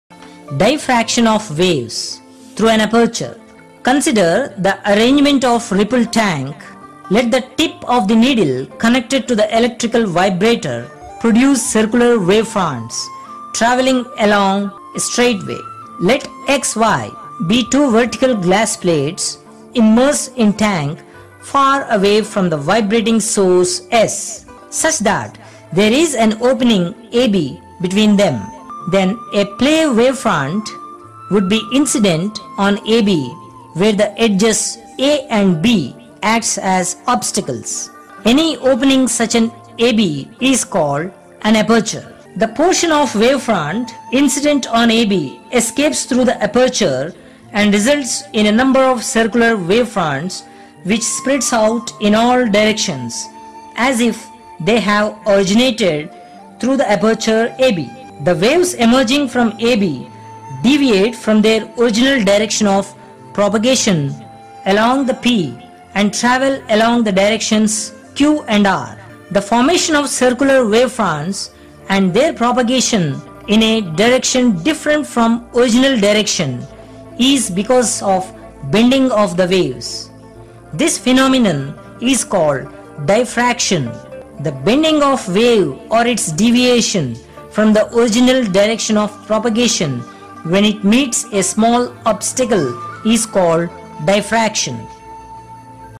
Diffraction of Waves | Class 10 Physics | Science | Digital Teacher
Learn about Diffraction of Waves of class 10 Science-Physics exclusively on Digital Teacher.